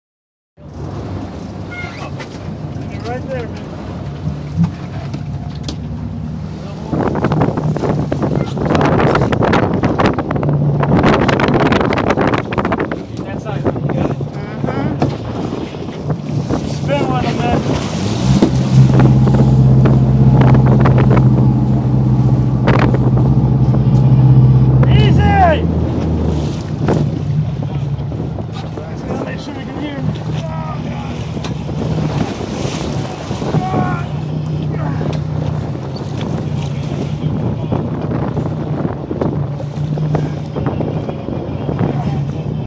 14 ft Tiger Shark deep sea fishing in Ft. Lauderdale. Either catch big fish or tell big lies!
Shark catch, est. 14 ft, 1,000 pounds, largest fish caught in 5 years! Deep sea fishing with fishingheadquarters.com w/ Capt. Adam and Mick